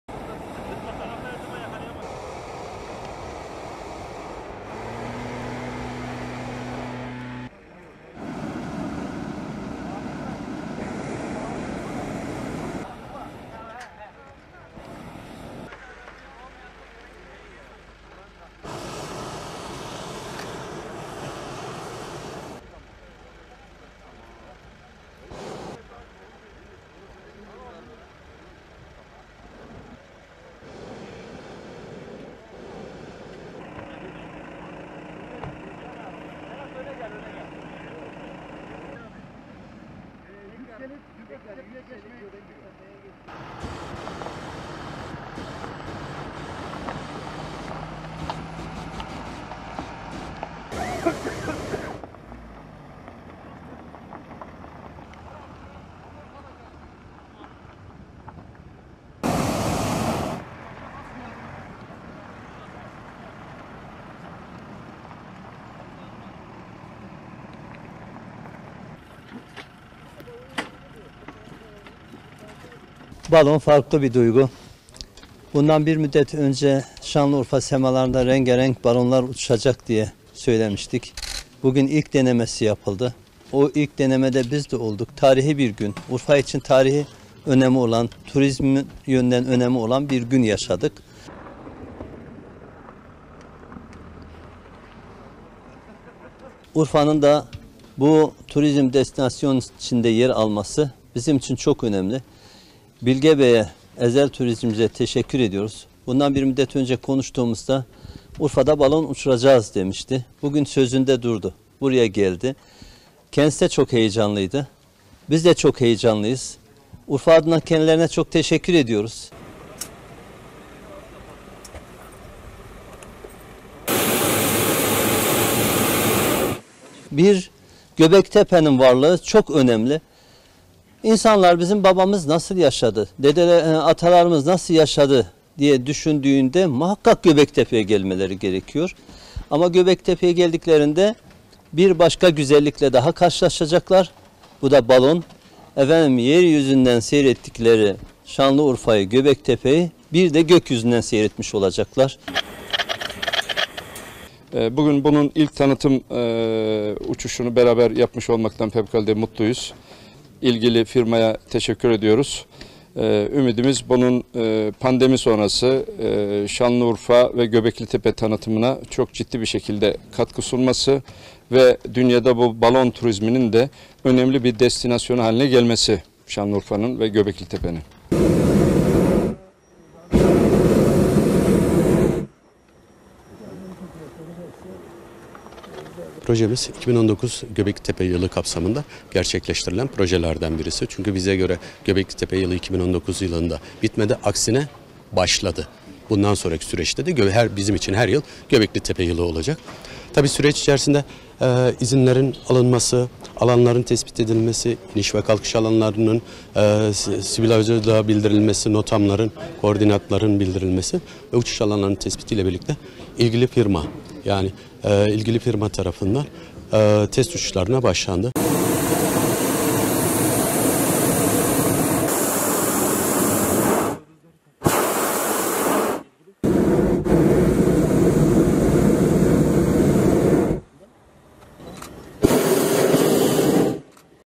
Göbeklitepe'de sıcak hava balonuyla tanıtım uçuşu gerçekleştirildi
UNESCO Dünya Mirası Listesi'nde yer alan ve "tarihin sıfır noktası" olarak nitelendirilen Göbeklitepe'de, bir süre sonra faaliyete başlayacak sıcak hava balonunun tanıtım uçuşu gerçekleştirildi. Sınırlı sayıda kişi ile 2 balonla havalanan katılımcılar Göbeklitepe'yi gökyüzünden izleme fırsatı buldu.